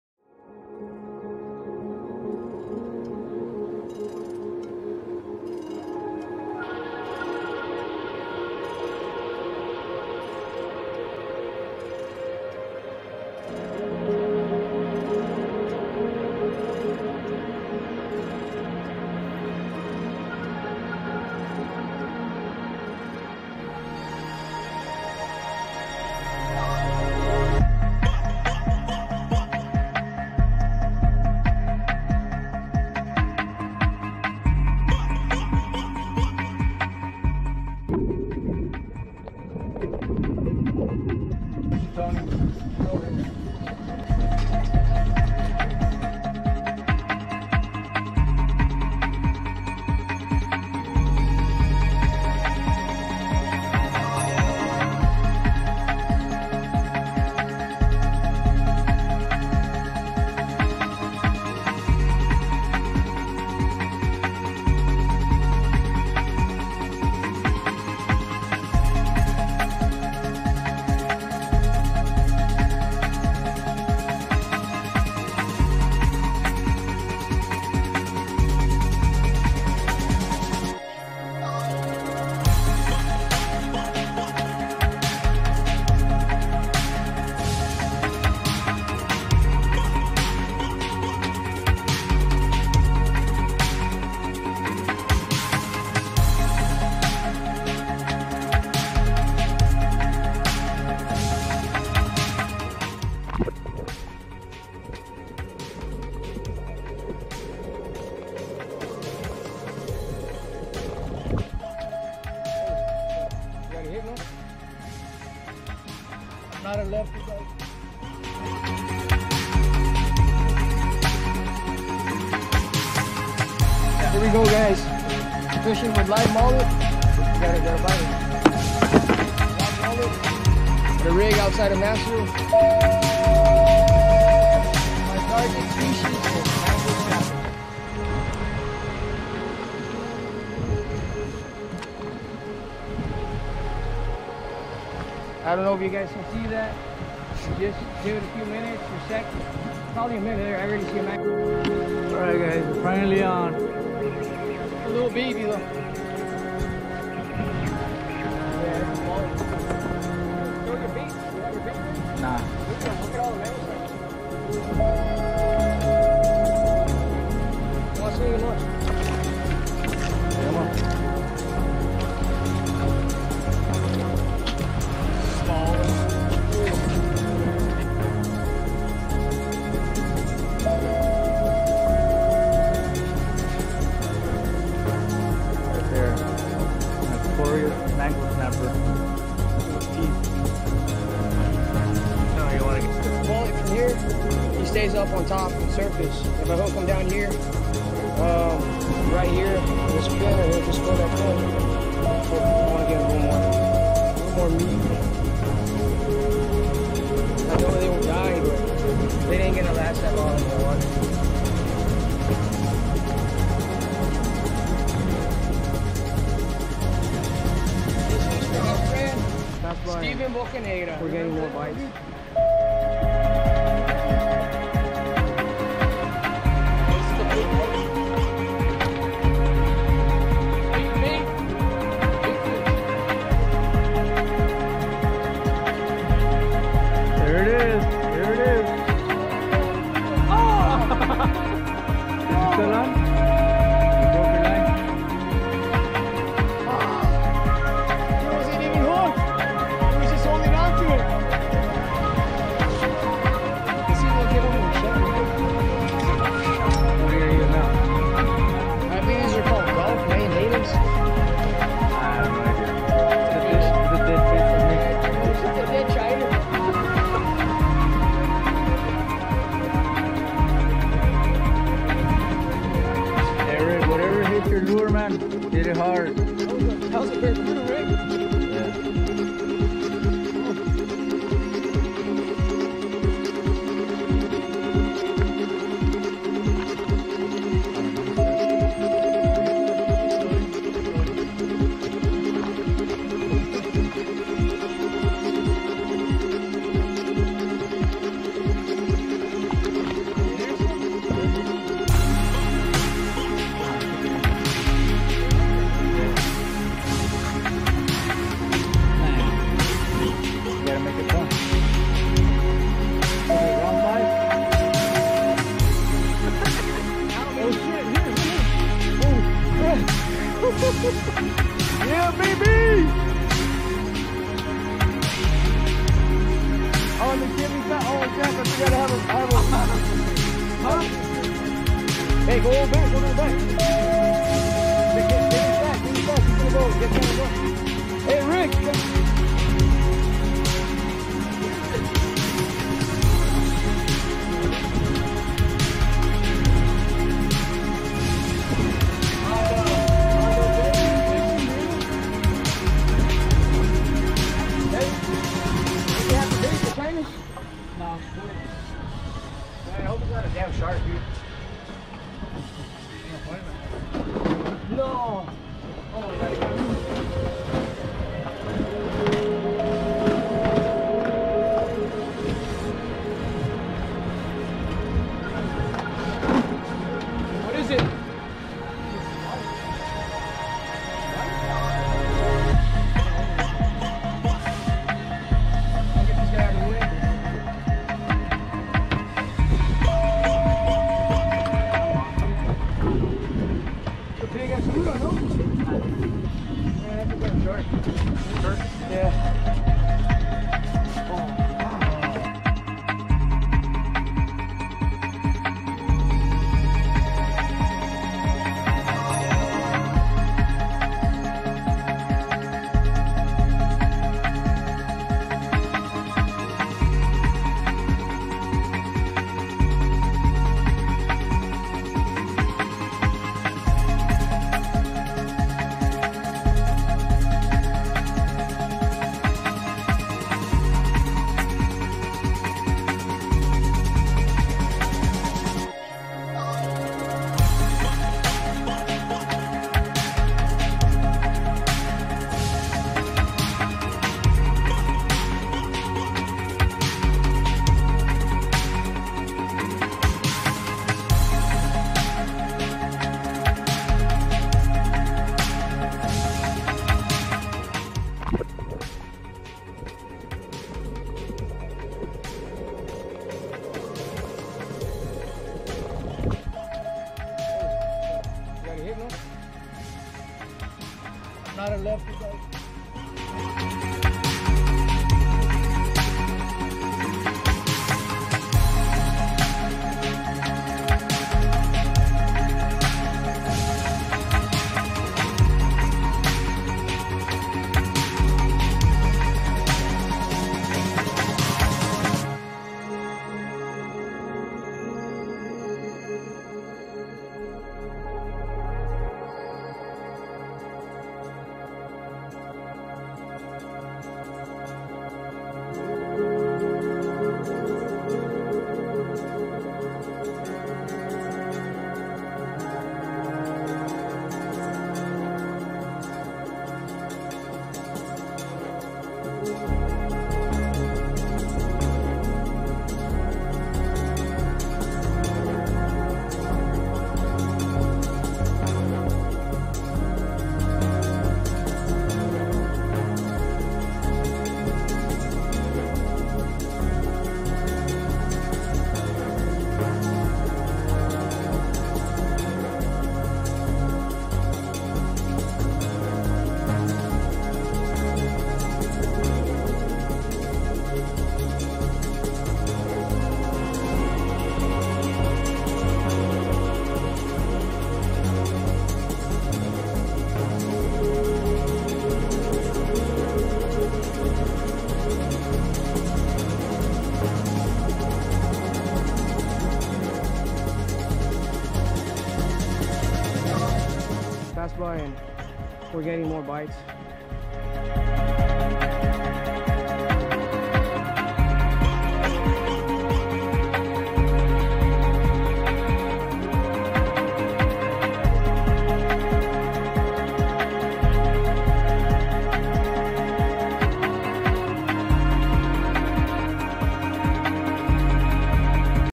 Port Mansfield Rig Fishing Trip Mahi, Monster Mangroves, Red Snapper EXTENDED Version
Hey Guys this is the more complete version of the previously posted video of the offshore trip the KillinTime Crew did. More videos to come!